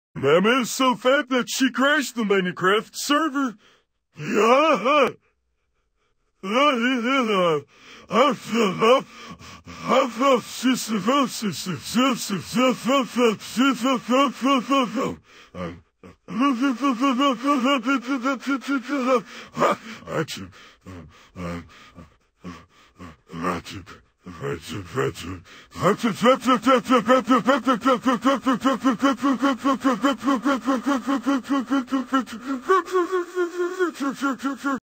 patrick star tells a joke, has a stroke, becomes a helicopter and discovers the true meaning to life
Backstory: one of my friends put in this line of text into a text-to-speech thing: Yo mama is so fat, that she crashed the Minecraft server! GAHAHAHAHAHA
And apparently Patrick could simply not handle laughter